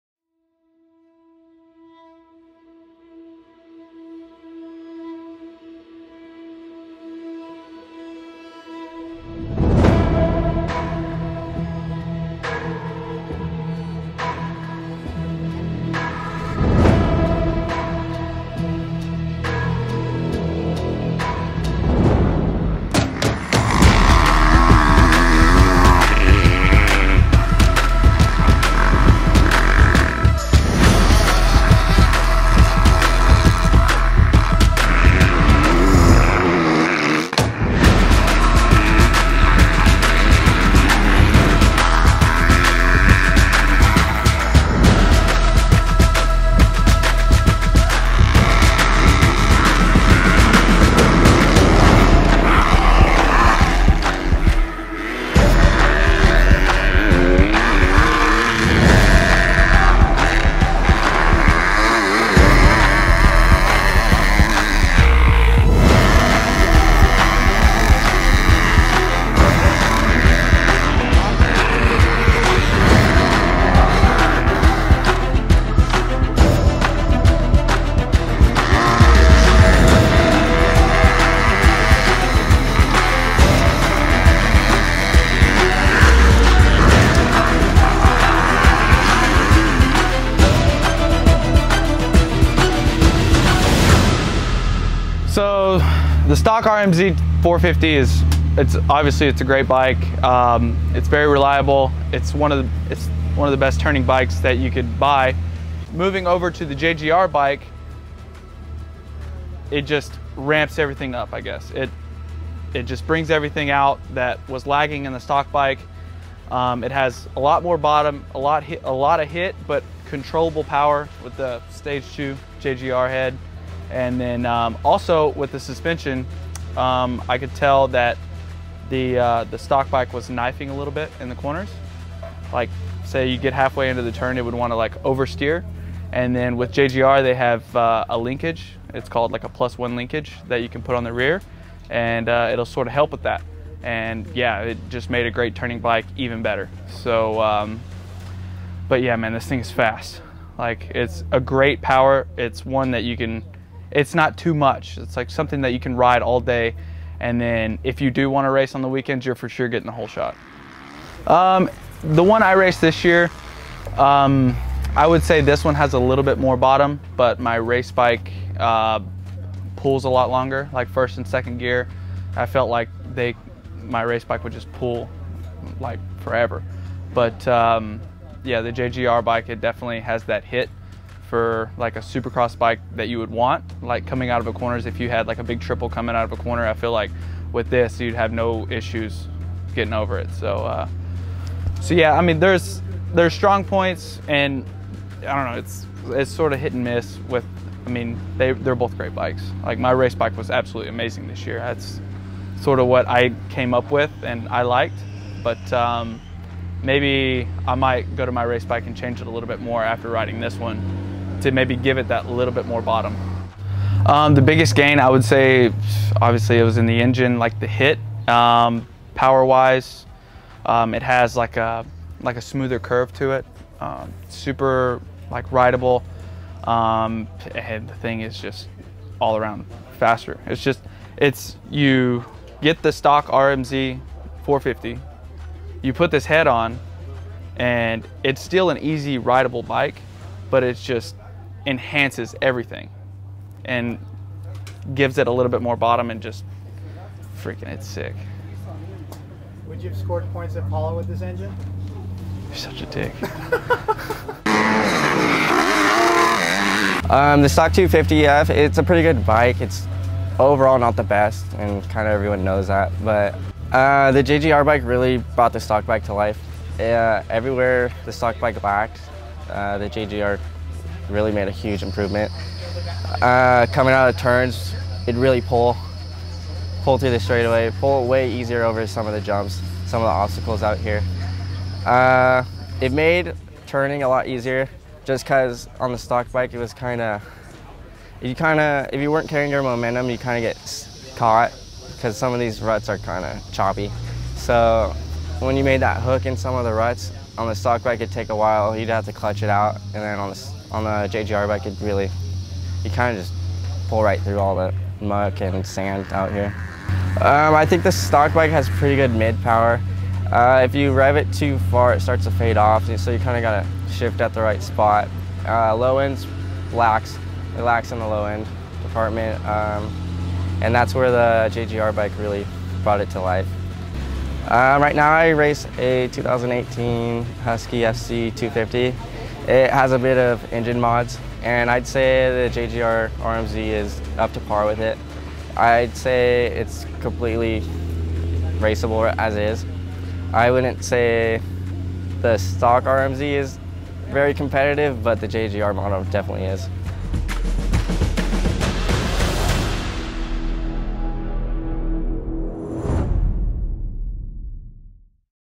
JGR Suzuki Performance Packages Ride Test with Alex Ray and Cody Whitsett
Yesterday, JGR and Suzuki invited Swapmoto Live out to Perris Raceway to test a pair of Suzuki RM-Z motocross bikes that were outfitted with the JGRMX Phase Two performance engine packages. We brought out Alex Ray and Cody Whitsett to do the honors, and both were very impressed with the 250 and 450 mod bikes.